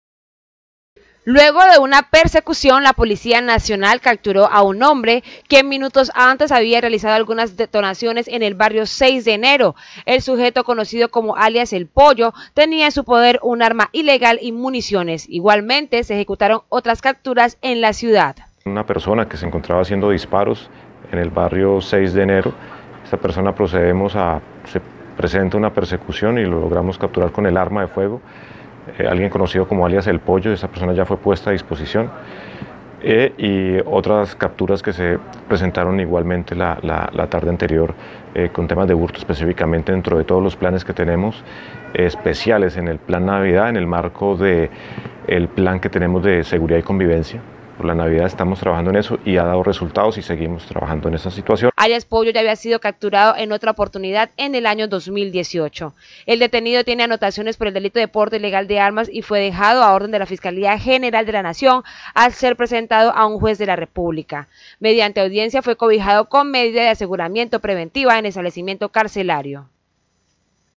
CAPTURADO ALIAS "POLLO"
La Policía Nacional capturó a un sujeto conocido con el alias de “pollo” después de haber realizado disparos, y por el delito de porte ilegal de armas y municiones.